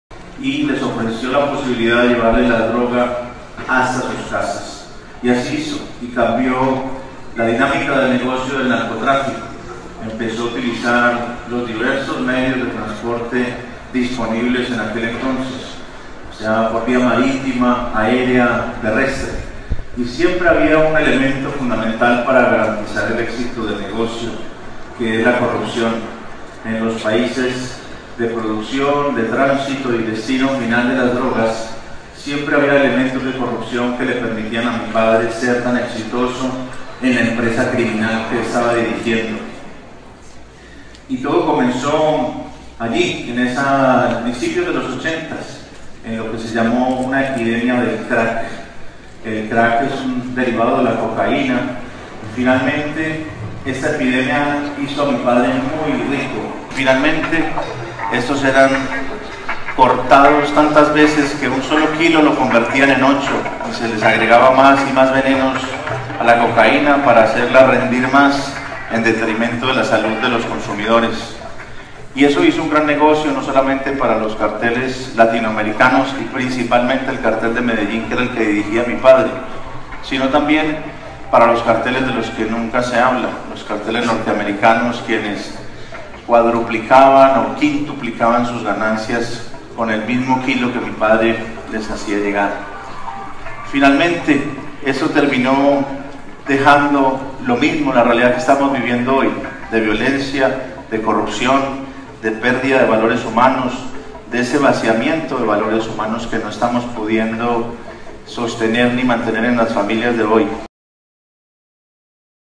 Pablo Escobar contribuye a la paz de Tamaulipas
En una conferencia en Matamoros ante más de tres mil estudiantes, Sebastián Marroquín, señaló que declararle la guerra a las drogas con ametralladoras, va en contra de los valores humanos.